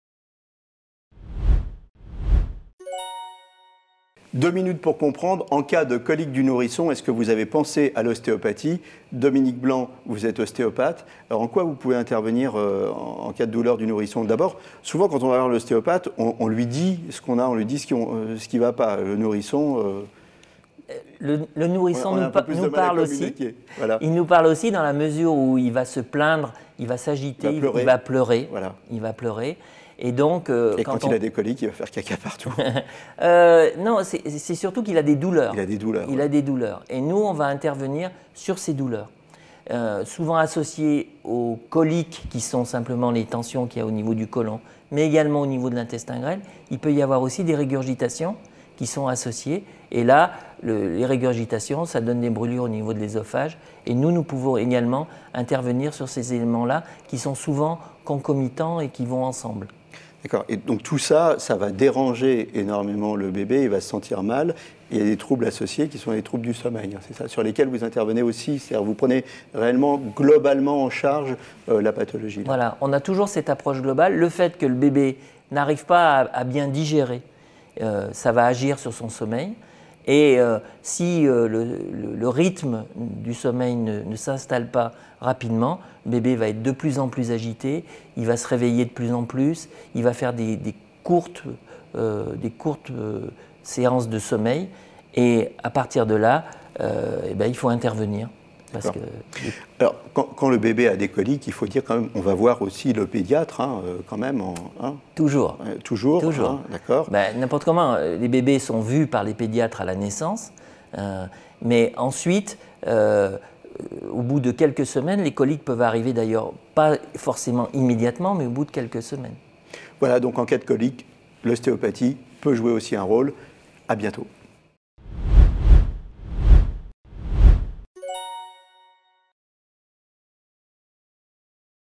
L'ostéopathie et les coliques du nourrisson
Dominique Blanc, ostéopathe est au micro de Jean-Claude Durousseaud Les coliques du nourrisson peuvent être soulagées par des séances d’ostéopathie. Le praticien agit sur le système digestif pour soulager le bébé et favoriser son sommeil.